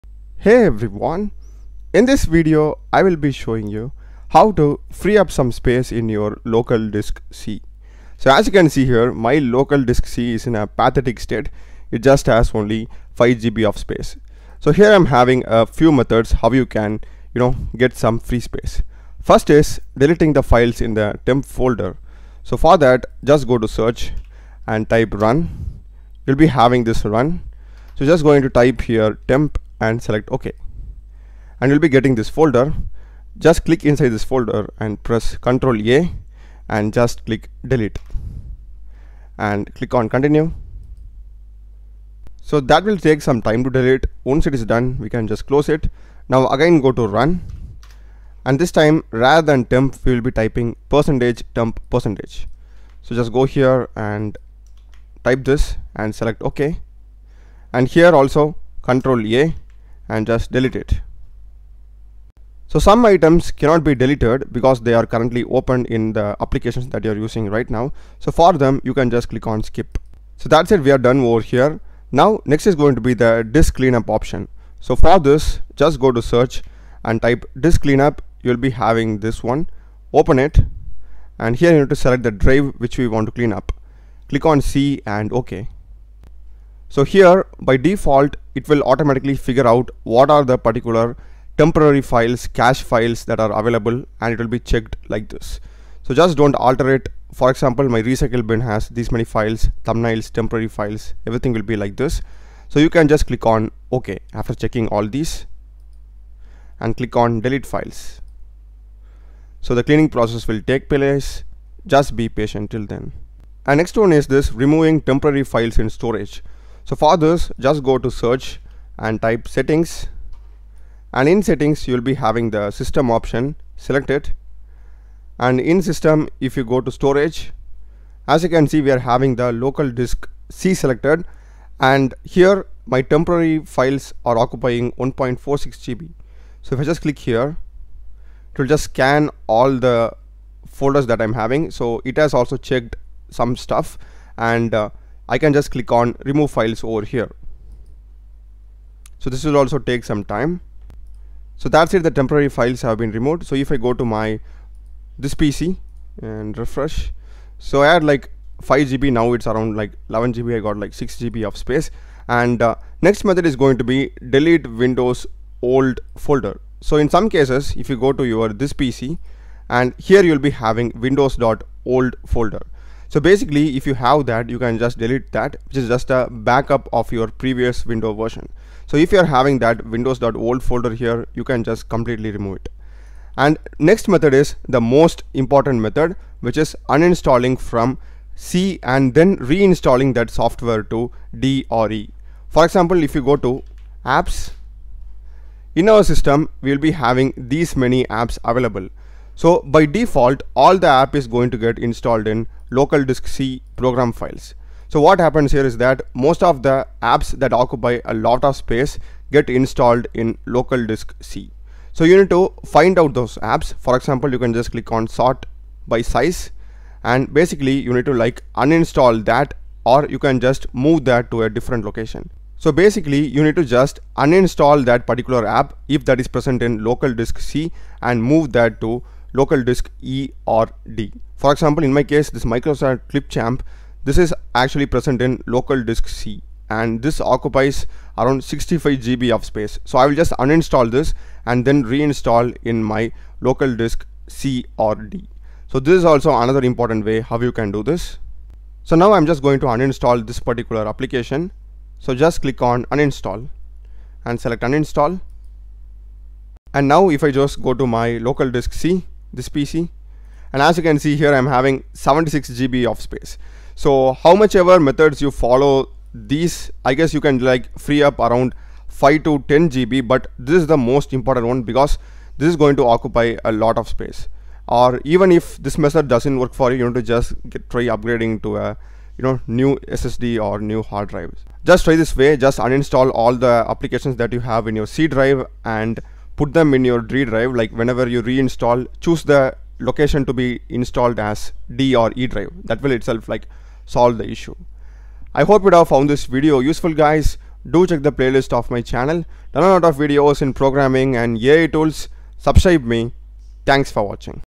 How to FREE UP Space in C Drive Windows 10 using 6 ways
In this tutorial we will see how to free up and make more space in local disk c drive in windows 10